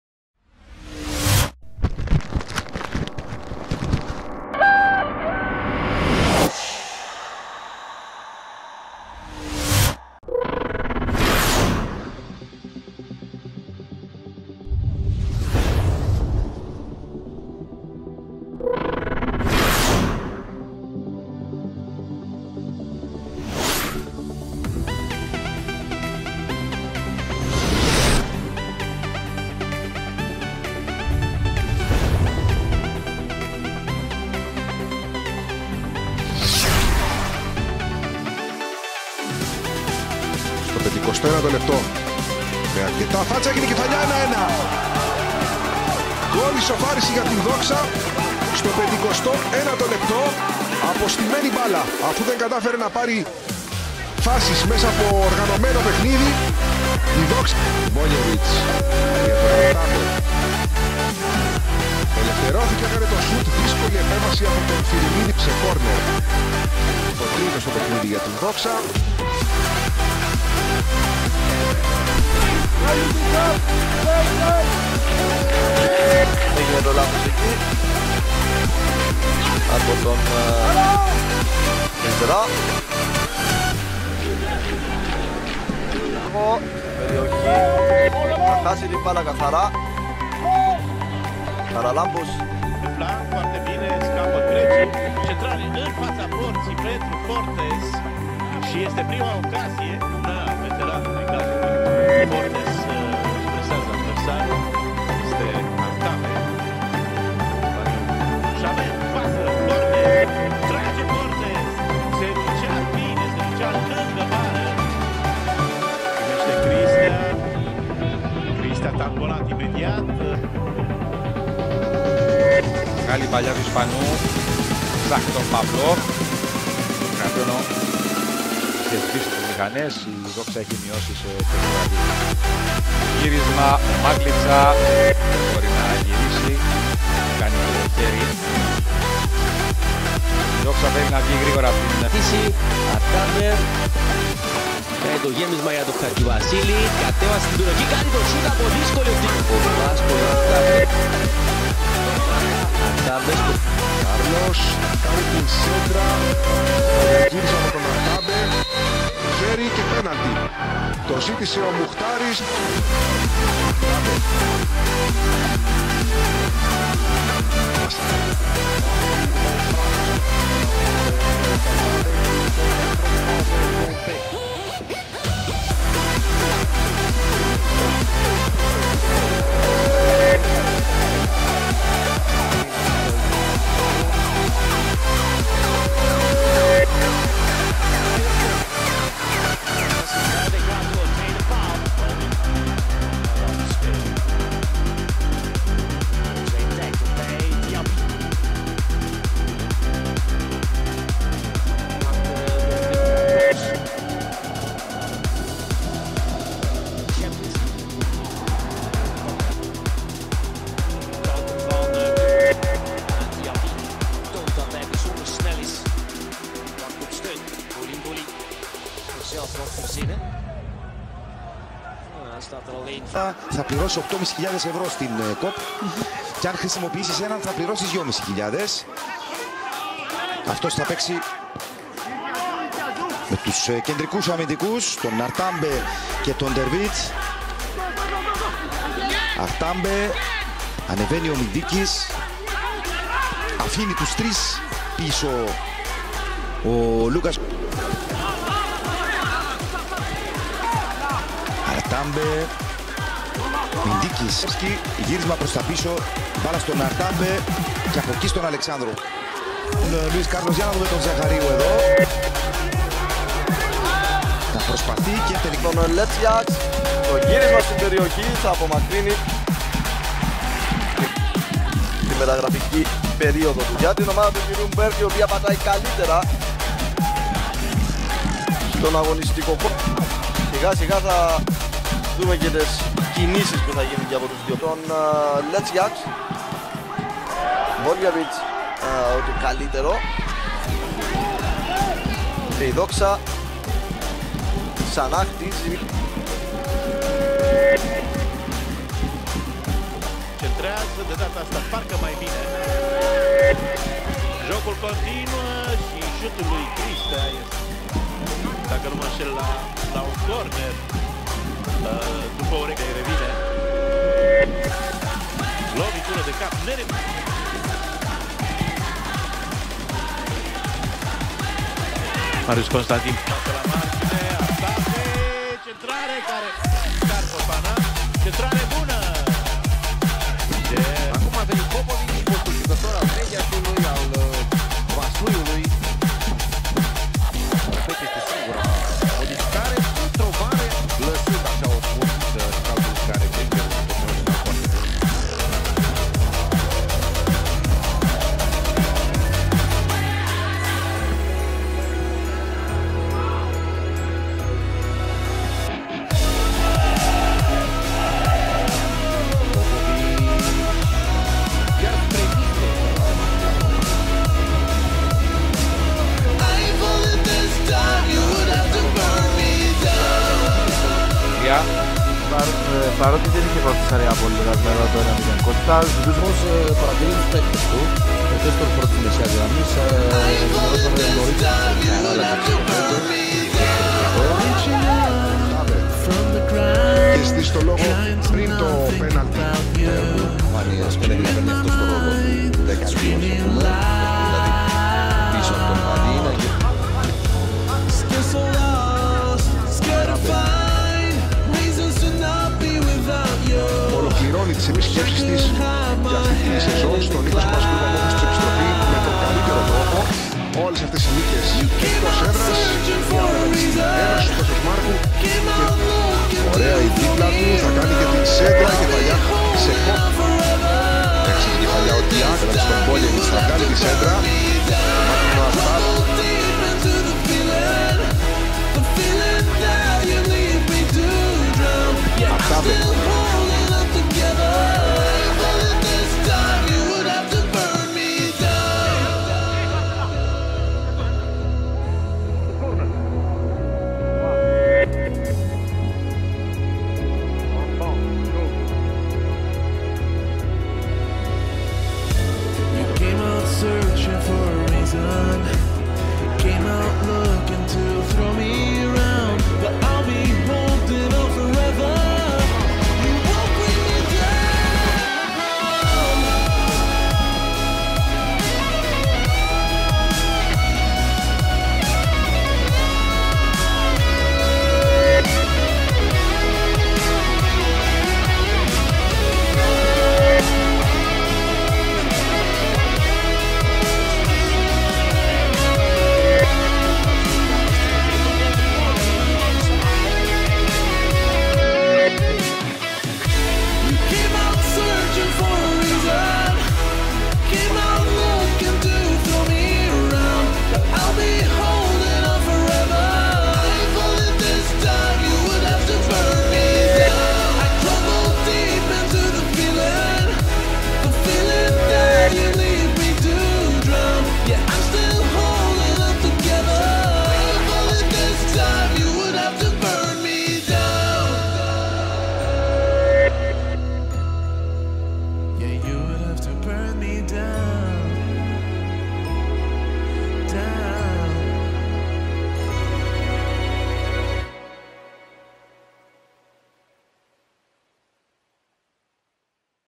Alfonso Artabe 2019
First name: Alfonso
Last name: Artabe Meca
Nationality: Spain
Date of birth: 18 August 1988
Country of birth: Spain
Place of birth: Palma de Mallorca
Position: Defensive Midfielder / Central Defender
Height:188 cm

[SUPERPOLLO DIGITAL SOLUTIONS]